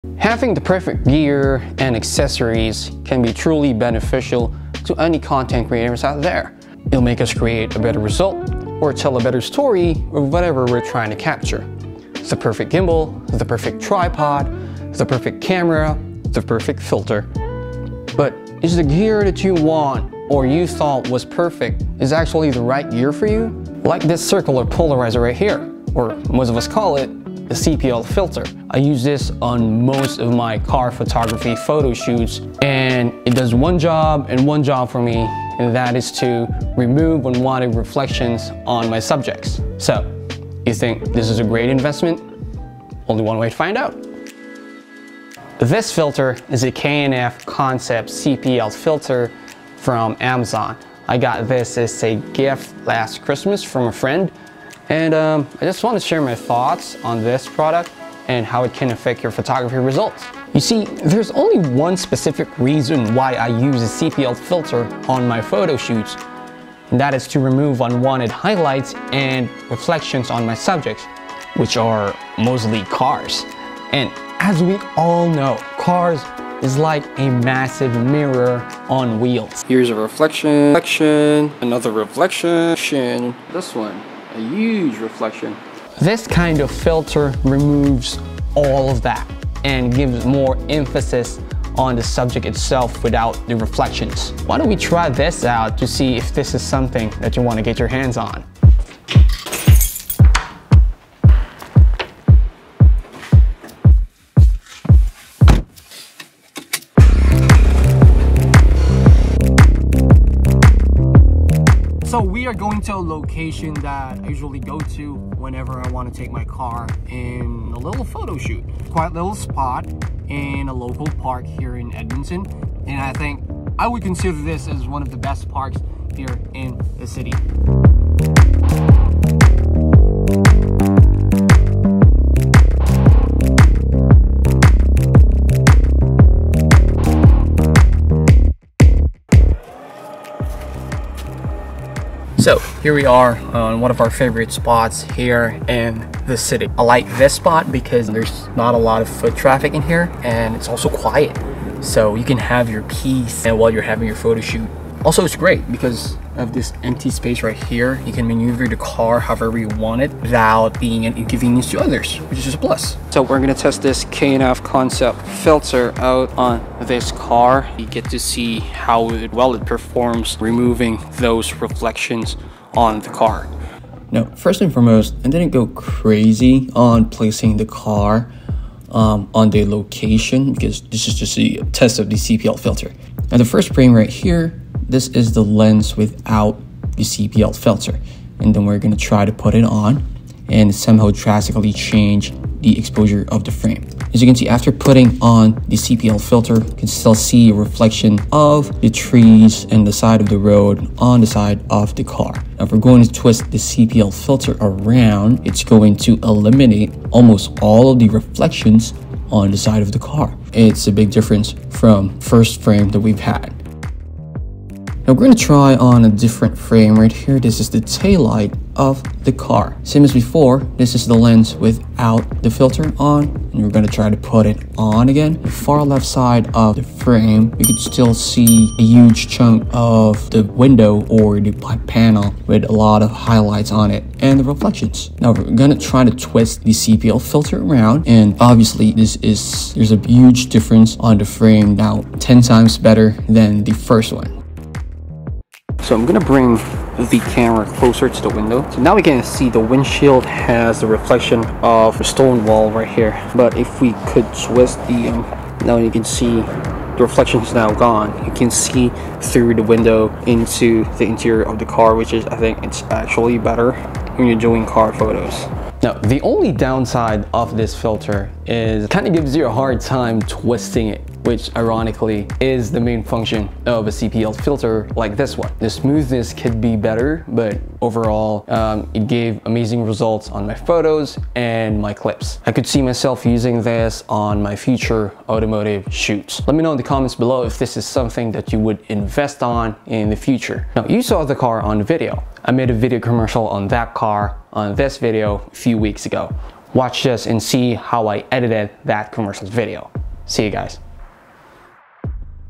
CPL Filter for Car Photography | K&F Concept CPL Filter Review
Circular Polarizer Filters is popularly used in car photography. Let's test out the K&F Concept Filter to see how it performs!